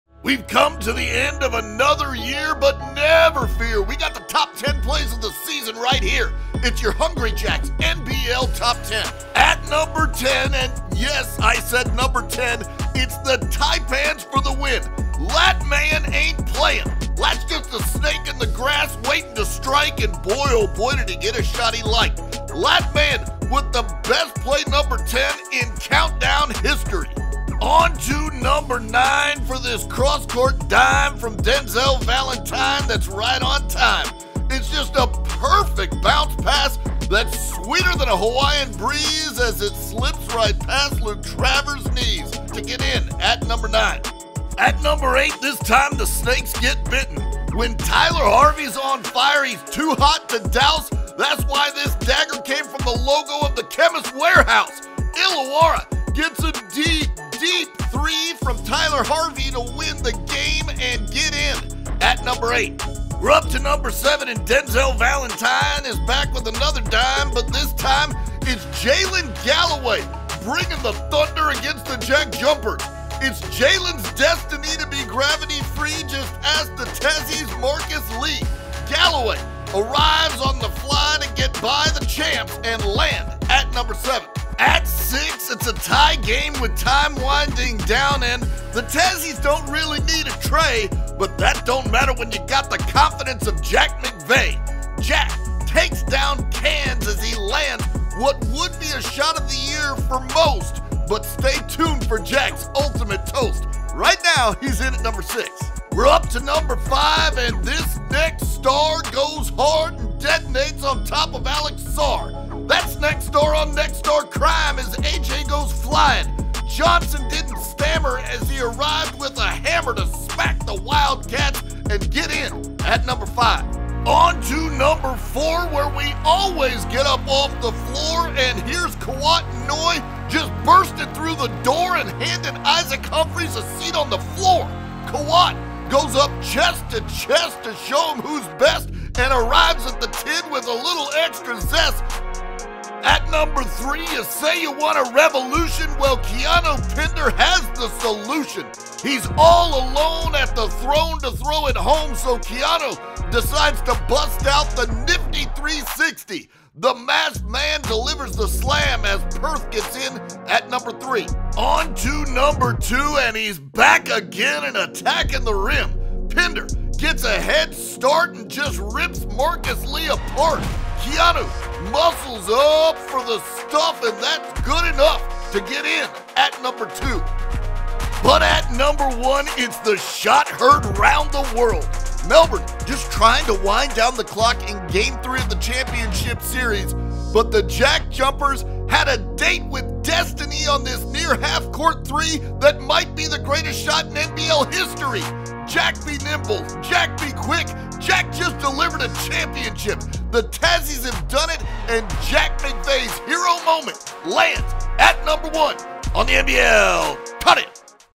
Top 10 Plays of NBL24
Counting down the Top 10 plays of the NBL24 season.

Want more NBL news and action ⤵️